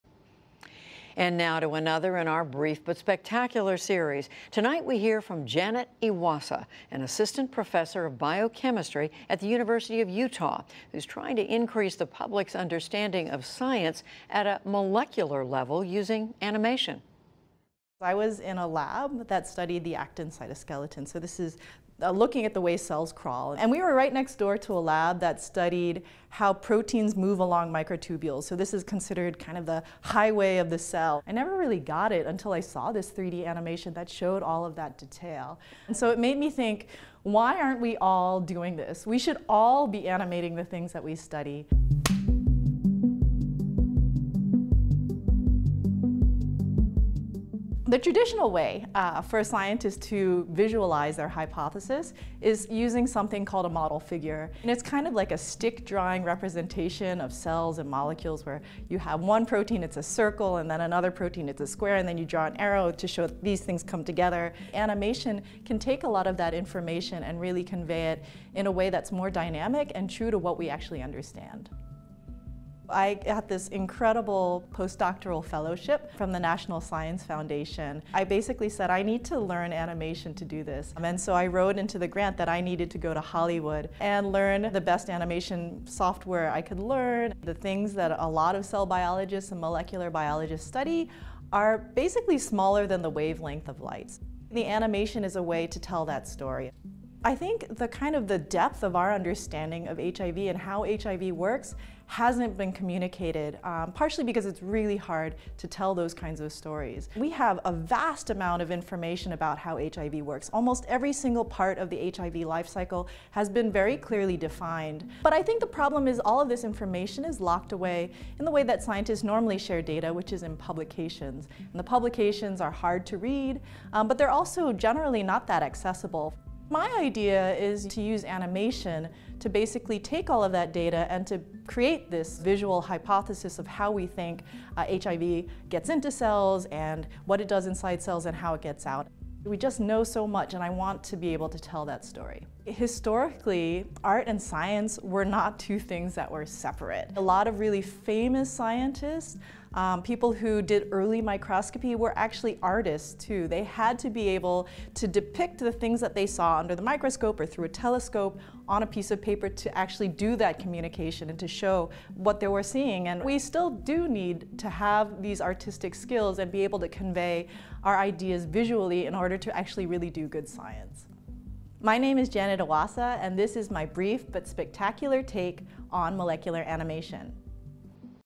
These 3D animations could help you finally understand molecular science
Art and science have in some ways always overlapped, with early scientists using illustrations to depict what they saw under the microscope. Janet Iwasa of the University of Utah is trying to re-establish this link to make thorny scientific data and models approachable to the common eye. Iwasa offers her brief but spectacular take on how 3D animation can make molecular science more accessible.